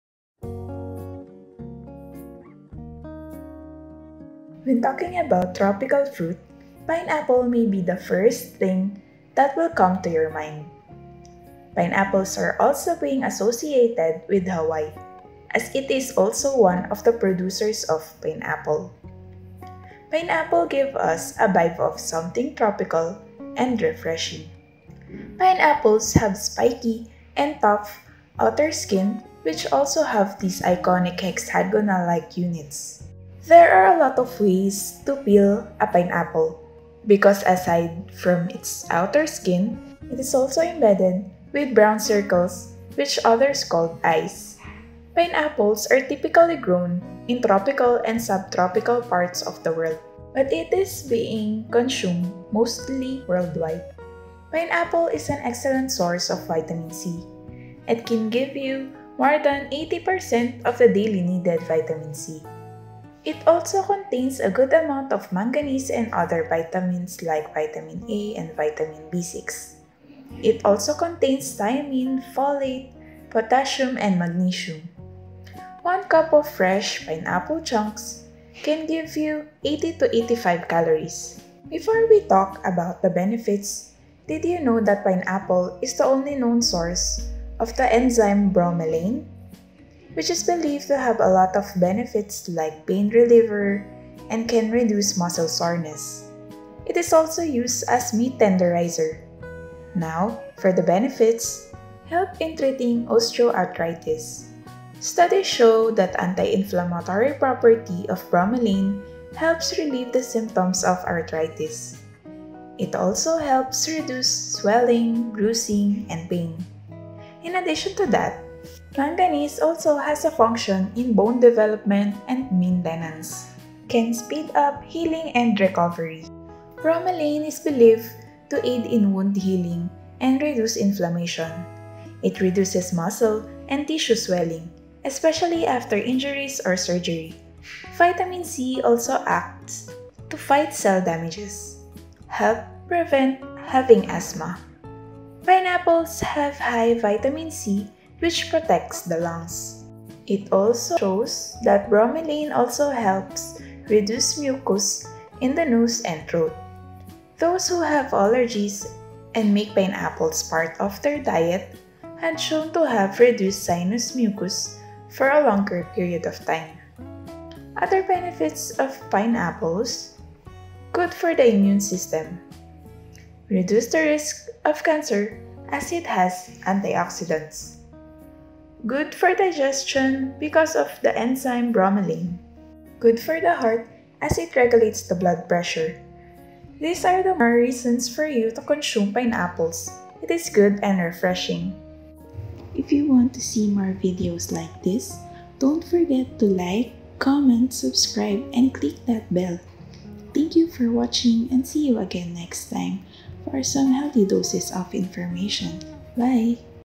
Pineapple: What it is? What does it contain? What are the benefits?
When talking about tropical fruit, pineapple maybe the first thing that will come to you mind. Pineapple gives us a vibe of something refreshing. In this video, we'll discuss the benefits of pineapple